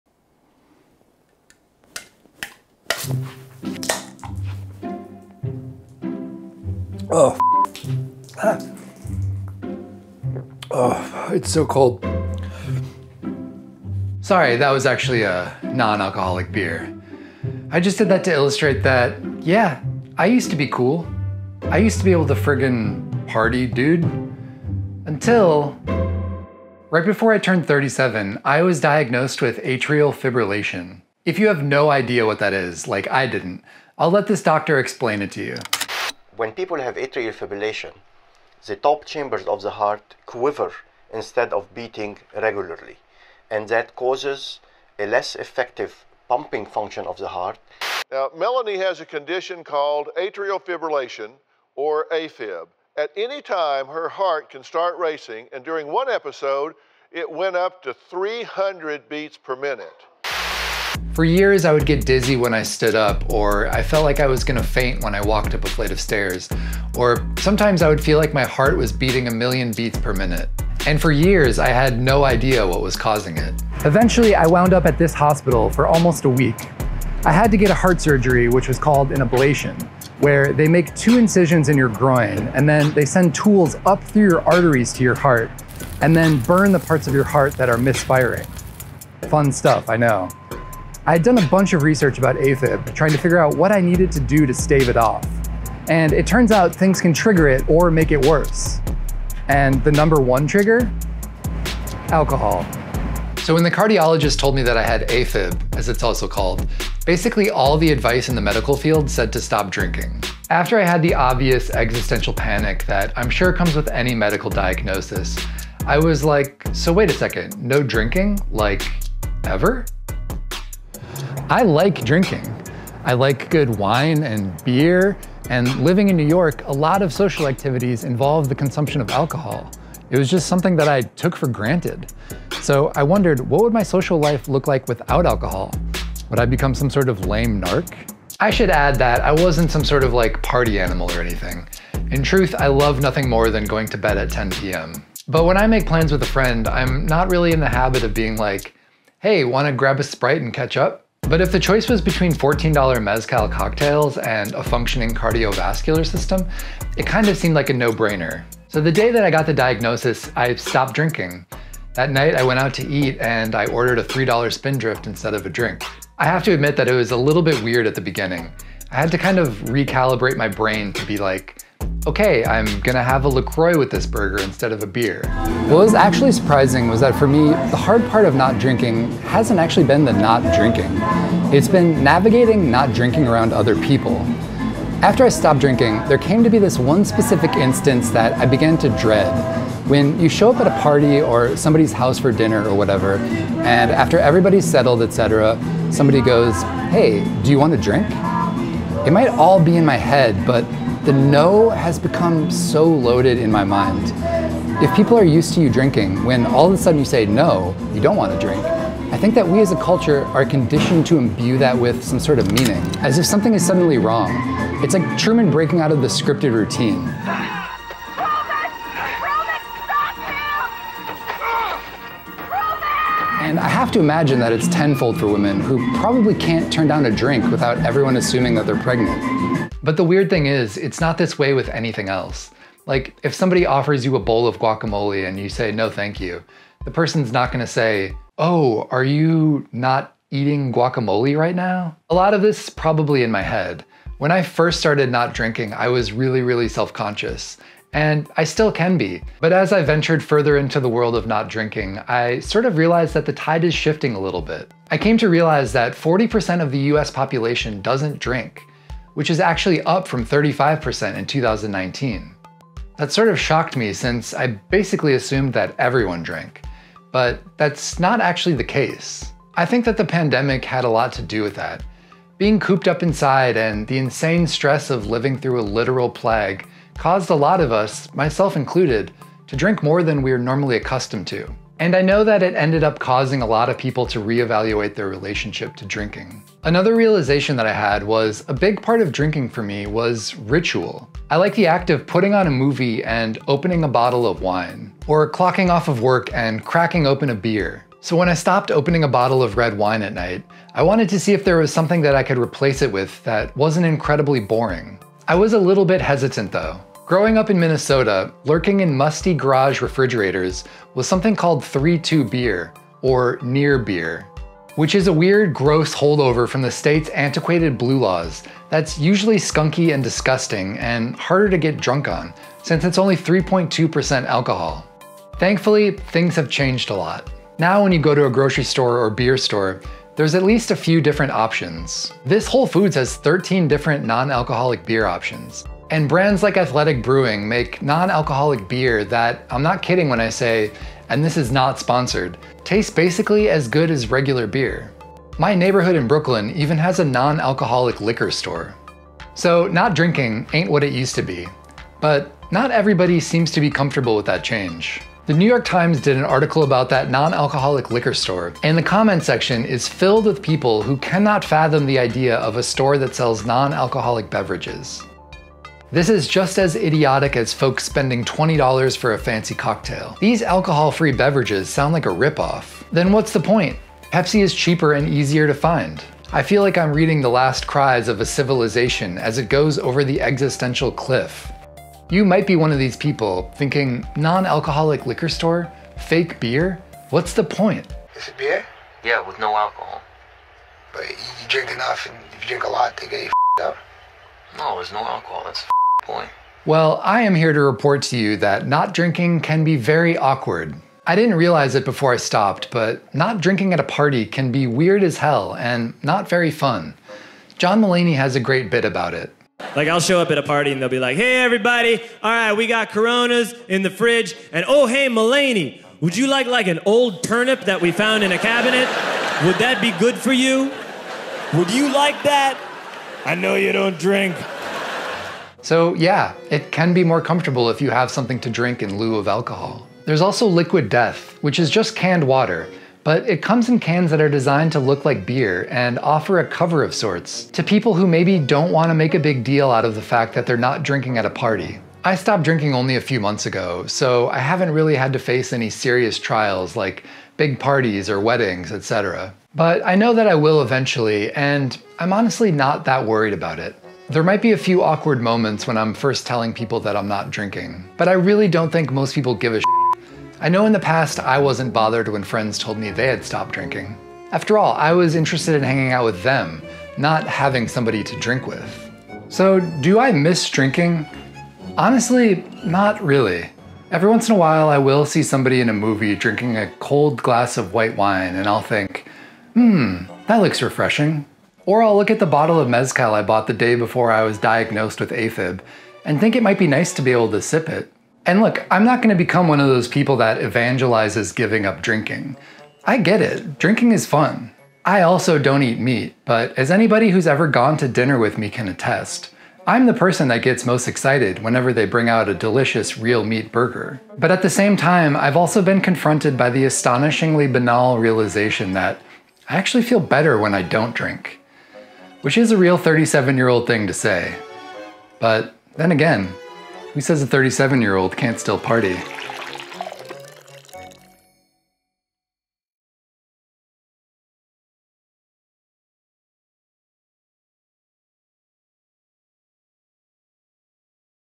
Why I gave up drinking
After being diagnosed with the heart condition atrial fibrillation I decided to quit drinking alcohol. The experience of not drinking booze was much different than I expected it would be. But thanks to a whole slew of new non-alcoholic beer brands and retail stores, being sober is a lot different than it used to be.